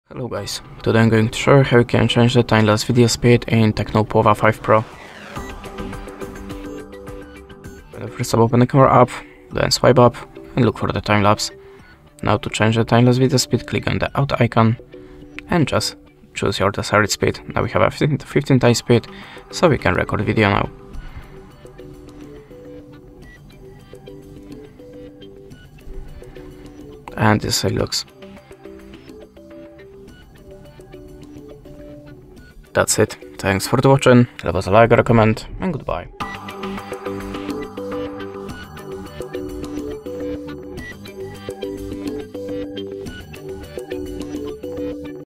How To Change Time Lapse Video Speed On Tecno Pova 5 Pro
Unleash your creativity with time-lapse videos using this guide on adjusting time-lapse video speed on your Tecno Pova 5 Pro. We'll provide you with step-by-step instructions on how to control the pace of your time-lapse recordings, allowing you to create captivating visual stories. Whether you want to speed up the action or slow down time for dramatic effect, this guide empowers you to make your time-lapse videos truly dynamic in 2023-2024.

How can I change the time-lapse video speed on my Tecno Pova 5 Pro?
Why is it important to adjust time-lapse video speed, and how does it impact the final result?
Are there specific settings or options for controlling the speed of time-lapse videos on the device?
Can I change the speed after recording, and how does it affect the video's duration and quality?
Will altering the time-lapse video speed impact the device's performance or storage space?